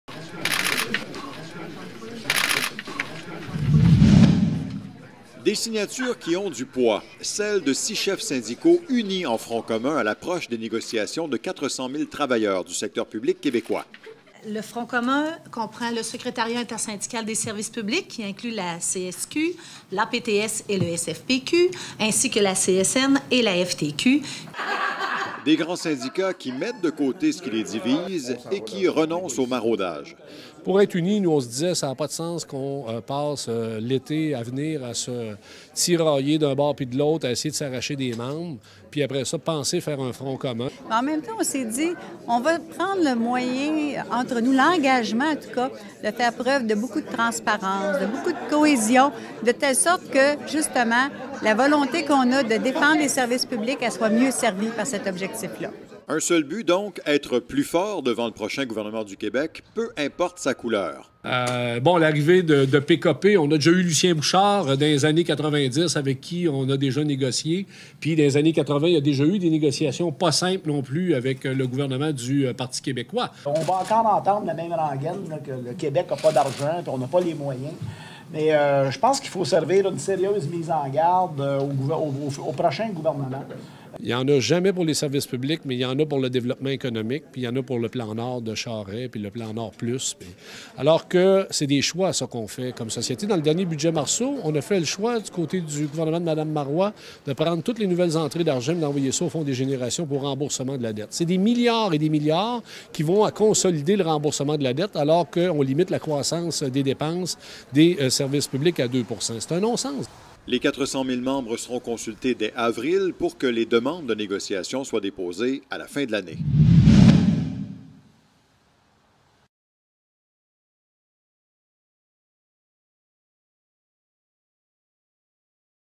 Négo 2015 : Formation du Front commun
Les grandes organisations syndicales se réunissent pour former un front commun syndical en vue de la négociation du secteur public 2015. Cette vidéo n'a pas été produite spécifiquement dans le cadre de la campagne « AVEC nous », mais elle en marque en quelque sorte le début.

Publication originale : 16 mars 2014